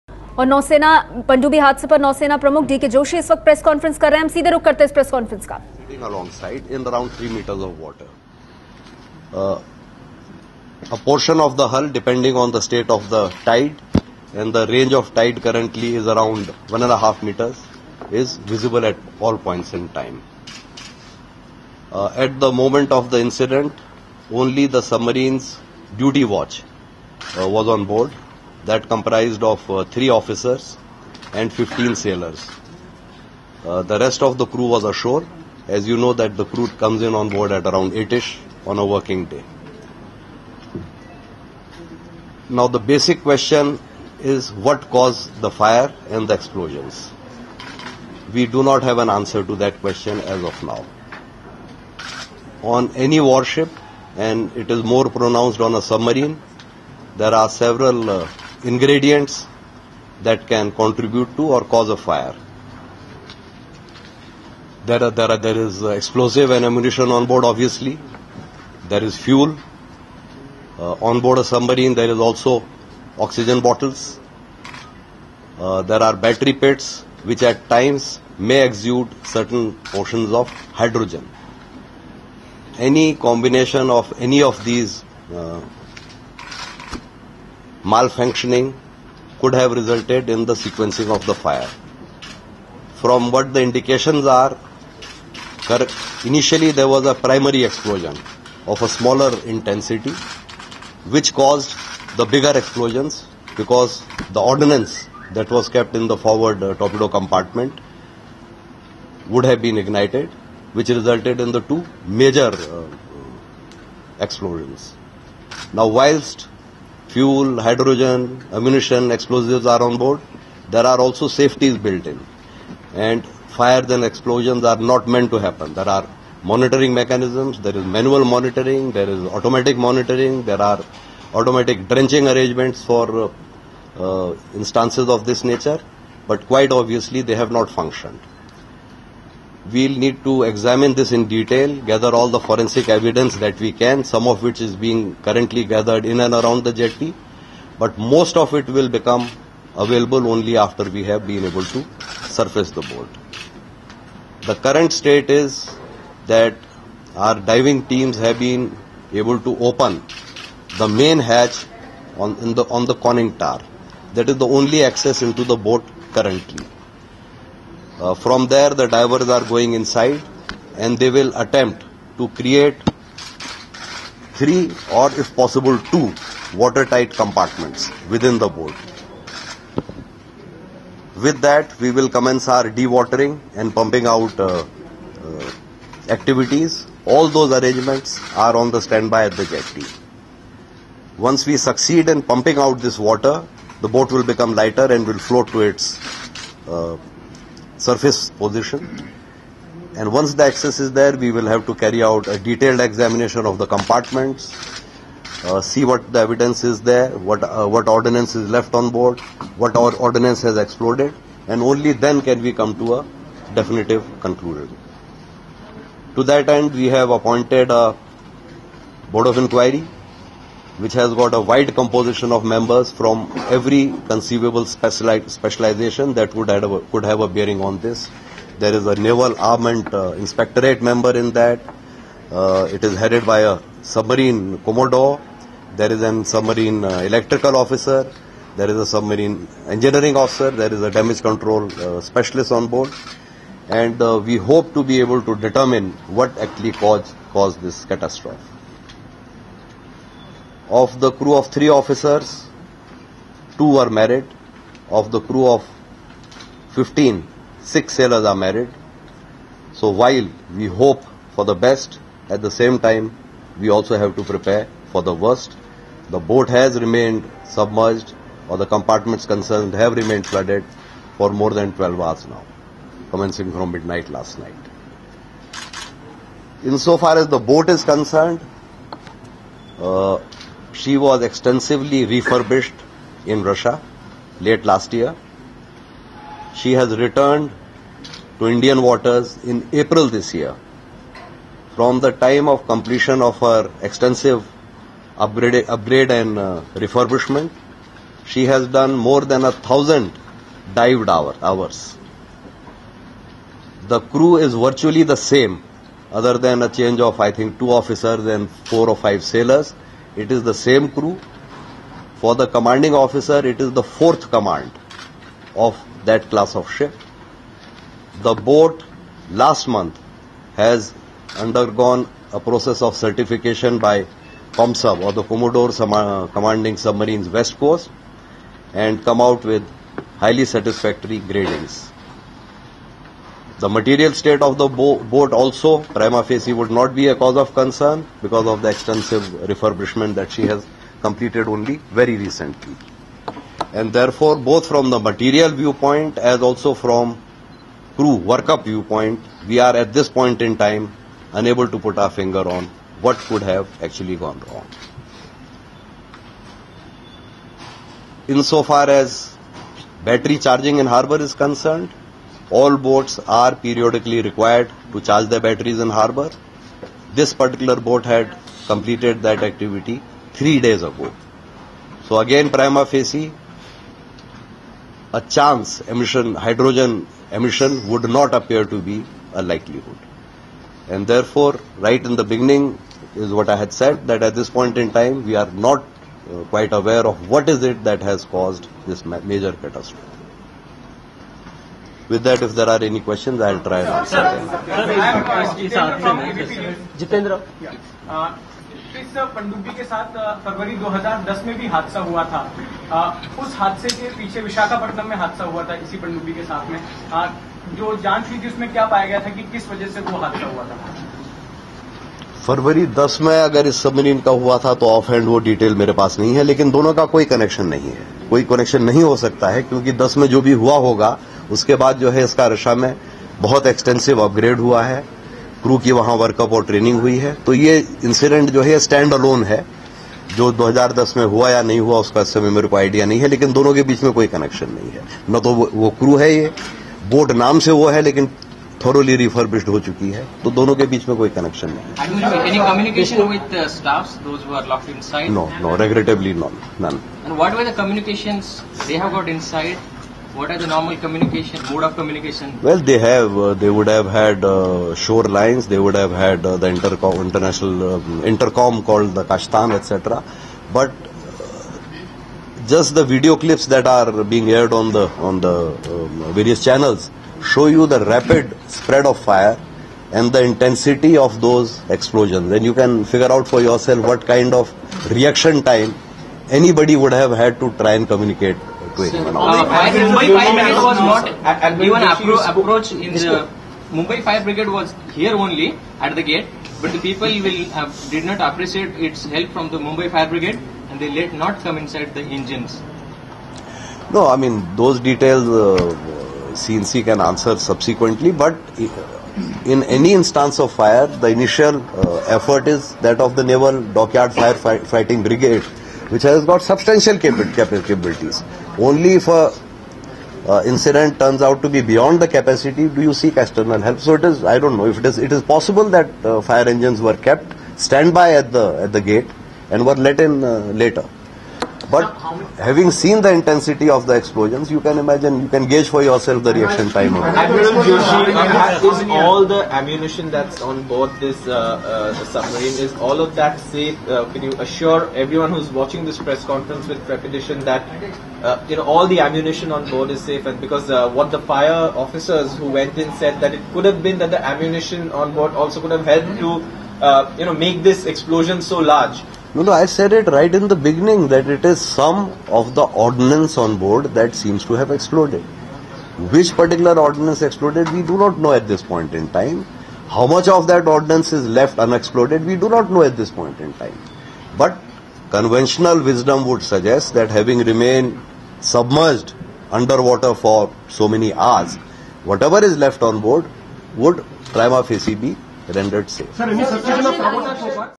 It may not be sabotage: Naval chief DK Joshi
The Indian Navy chief said on Wednesday that sabotage was not ruled out behind the sinking of a submarine here following a huge explosion but it was highly unlikely.